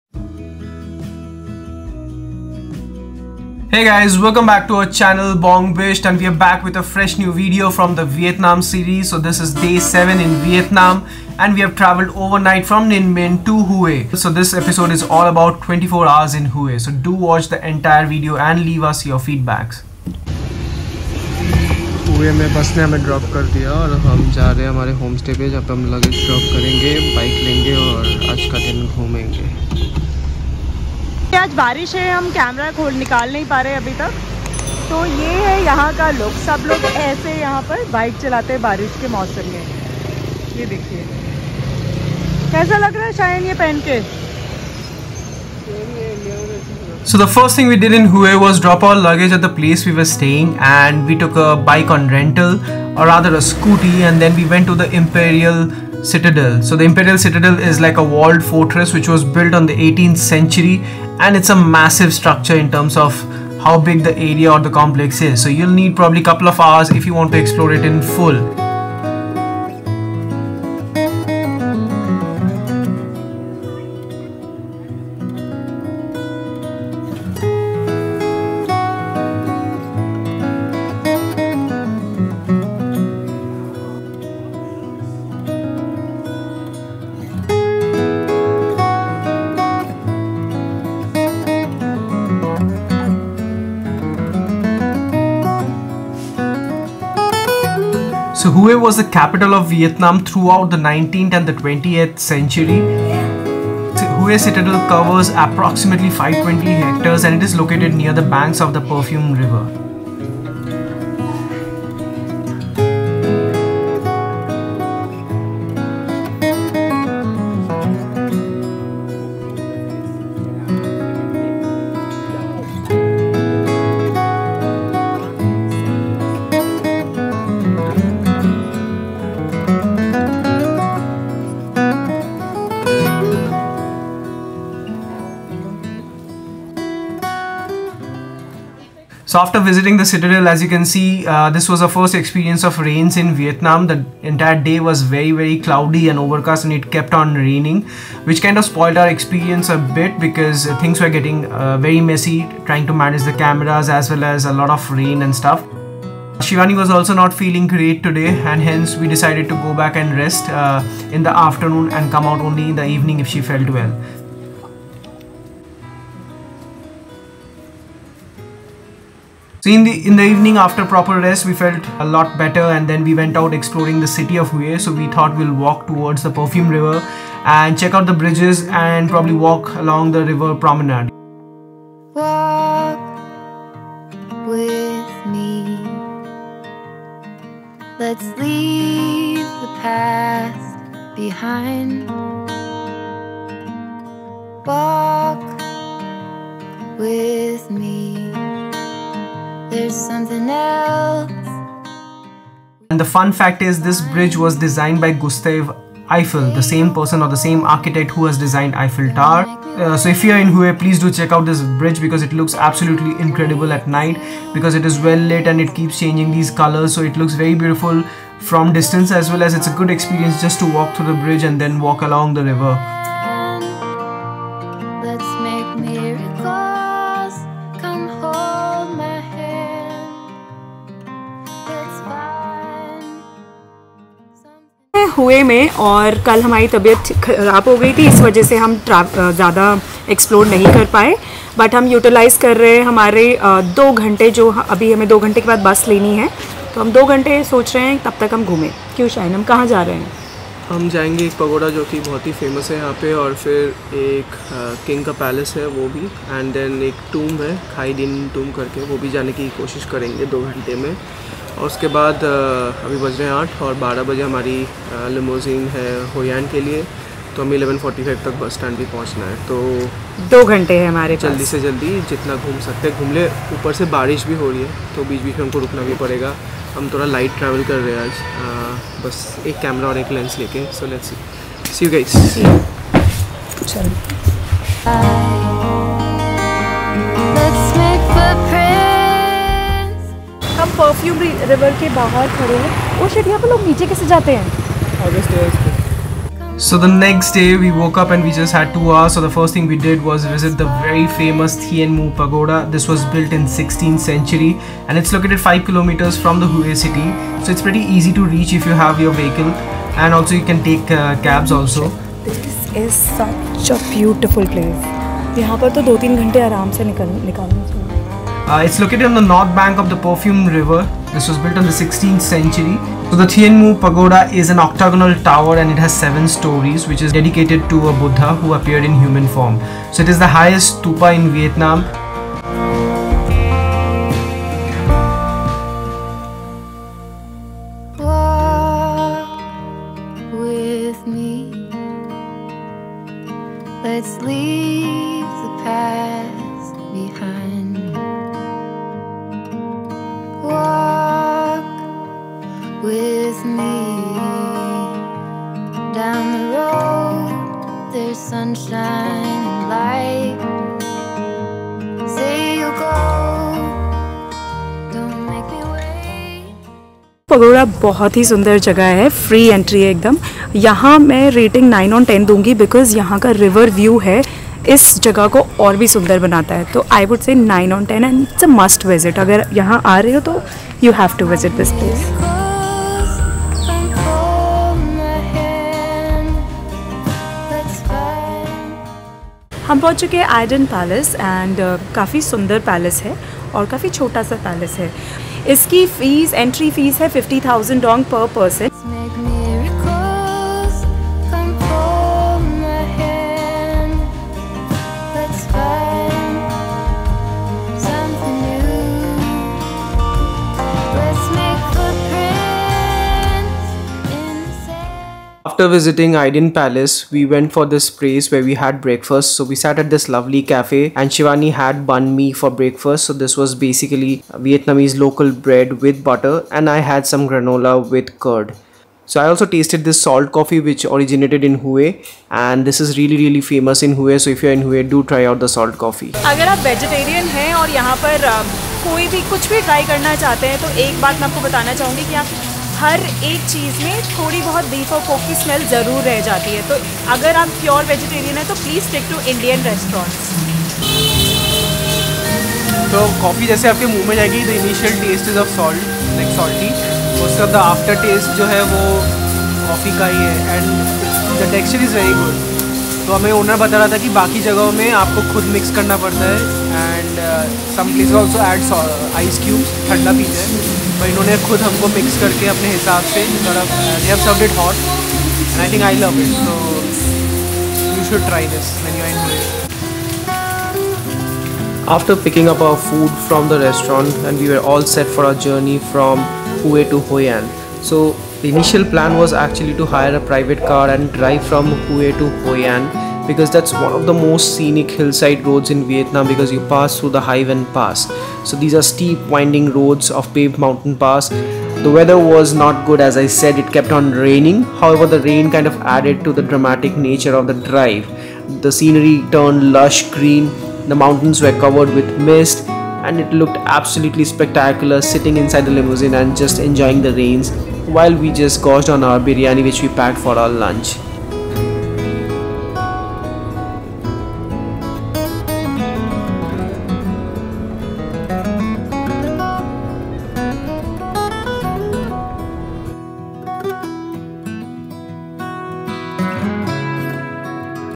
HUÉ VIETNAM - Vietnam’s ancient capital city | VLOG 2022 | EP7 VIETNAM
Welcome to our exciting adventure in Huế, Vietnam, where we'll spend an incredible 24 hours exploring some of the city's most iconic landmarks! Join us as we take you on a captivating journey through the rich history and breathtaking beauty of Huế.

🏰 Imperial Citadel:
Our first stop is the majestic Imperial Citadel, a UNESCO World Heritage site. Step into the past as we wander through the ancient walls of this imperial palace complex, once home to the Nguyen Dynasty. Discover the intricate architecture, stunning gardens, and learn about the fascinating history that unfolds at every turn.

🌉 Trường Tiền Bridge:
Next, we make our way to Trường Tiền Bridge, an iconic symbol of Huế. As we walk across this enchanting bridge, we'll witness the mesmerizing view of the Perfume River and the city skyline. Breathe in the fresh air, take in the vibrant atmosphere, and capture some incredible photos along the way.

🕌 Thiên Mụ Pagoda:
Prepare to be amazed as we visit Thiên Mụ Pagoda, one of the oldest and most beautiful pagodas in Vietnam. Nestled on the banks of the Perfume River, this seven-tiered pagoda boasts stunning architecture and breathtaking views of the surrounding landscapes. Immerse yourself in the tranquility and spirituality of this sacred place.

🏛 Ai Đình Palace:
Our final destination is the grand Ai Đình Palace, a magnificent structure built as a mausoleum for Emperor Khải Định. Marvel at the intricate details of the palace's design and explore the fascinating exhibits that showcase the opulence of the Nguyen Dynasty. Learn about the life and reign of Emperor Khải Định as we delve into the historical significance of this splendid palace.

Join us on this incredible journey as we experience the wonders of Huế, from the ancient walls of the Imperial Citadel to the serenity of Thiên Mụ Pagoda. Get ready for a visual feast of history, culture, and natural beauty that will leave you longing to explore Huế for yourself!

If you want to skip to specific sections of the video just click on the chapter names and enjoy.

0:00 Intro
0:54 IMPERIAL CITADEL
2:50 TRUONG TIEN BRIDGE
4:50 THIEN MU PAGODA
6:27 AI DINH PALACE
6:56 BRUNCH
8:07 DRIVE TO HOI AN FROM HUE

Equipments we use for vlogging.

Vlogging Lens:

Zoom lens:

Cheers!
Shiwani & Shayan

Shiwani’s account: @BongBisht
Shayan’s account: @notsoshayan
Another account for travel: @oldagescrapbook